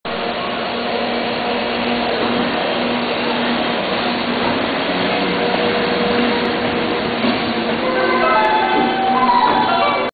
東海道の貨物2004 京浜東北線鶴見駅 EF64 1020単機が折り返し新鶴見方向へ発車
鶴見駅の海側構内は東海道貨物線を走行する列車の時間調整のための一時停車、機関車の付け替えによる貨物列車の方向転換にも使われる。また最近では団体専用などの臨時列車・回送列車車一時停車の場所としても使われる。